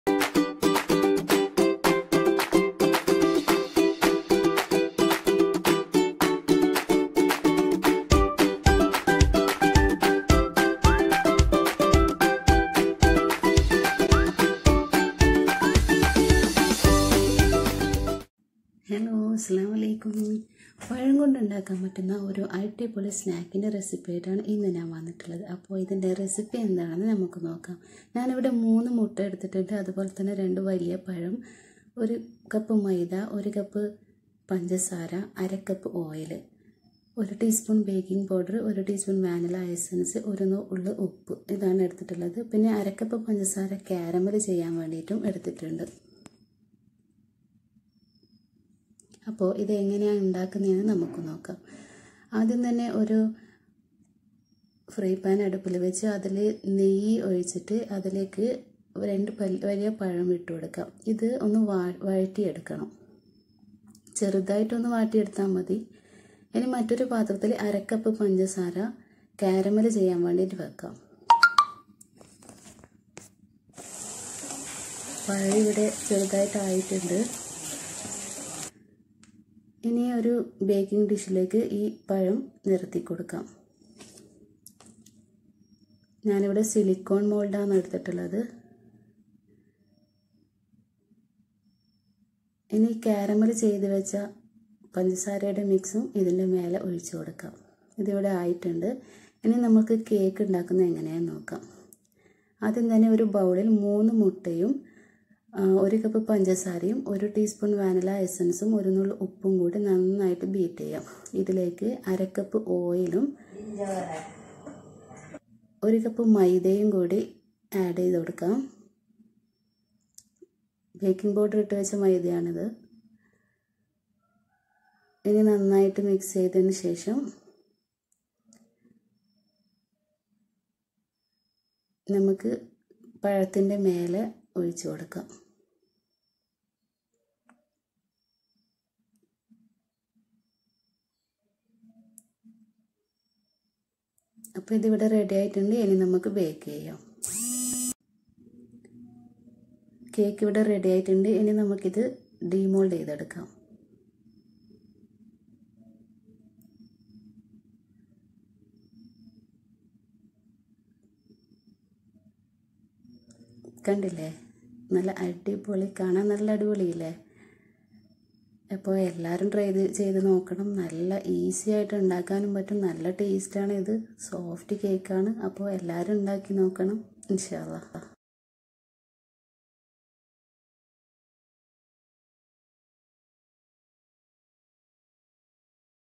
How To Make Banana upside down cake||Ifthar Recipe||Easy Snacks Recipe||Malabar tastes
INGREDIENTS

°banana 2
°egg 3
°flour 1cup
°sugar 1cup
°oil 1/2cup
°baking powder 1tsp
°vanilla essense1tsp
°Sugar 1/2cup(for caramel)